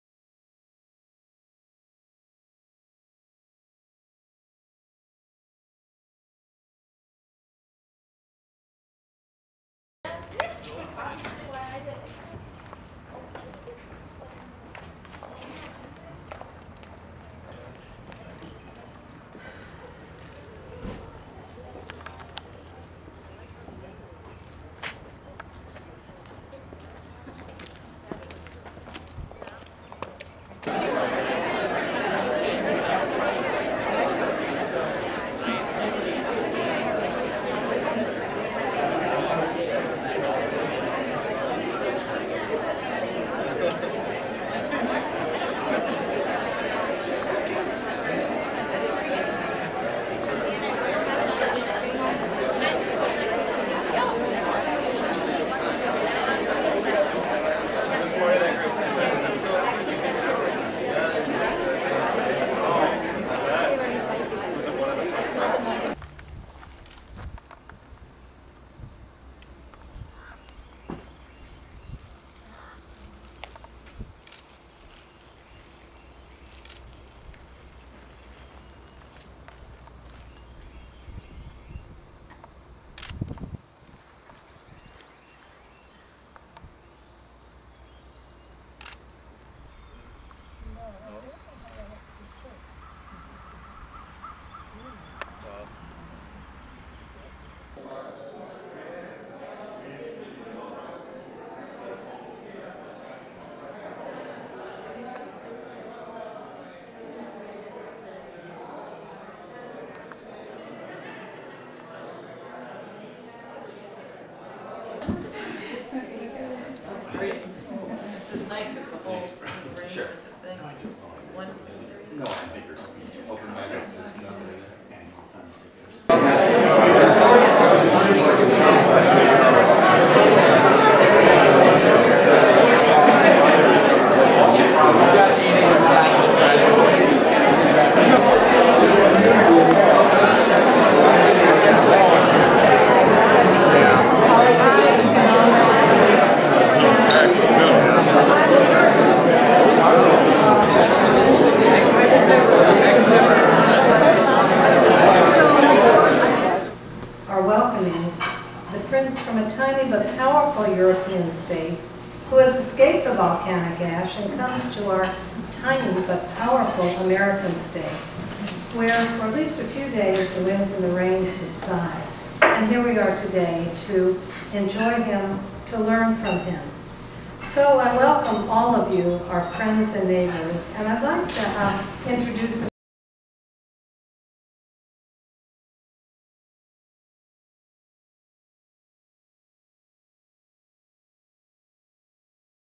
H.S.H Prince Alois of Liechtenstein visits Salve Regina 04/21/10
H.S.H Prince Alois of Liechtenstein tours the campus, speaks to the students, visits Sen. Pell's grave with Mrs. Pell and attends a luncheon in his honor at the Pell Center for International Relations and Public Policy